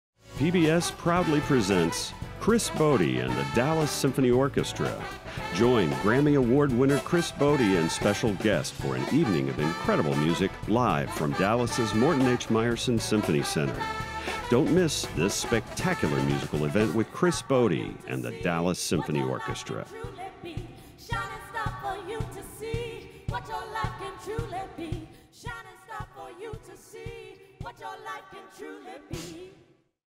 Chris Botti and The Dallas Symphony Orchestra [PROMO]
Join the master trumpeter and special guests for an evening of jazz, pop and favorite standards. Featuring violinist Caroline Campbell, guitarist Leo Amuedo, saxophonist Chad Lefkowitz-Brown and vocalists Sy Smith and Veronica Swift.
Watch Chris Botti and The Dallas Symphony Orchestra on Friday, August 19 at 8:00pm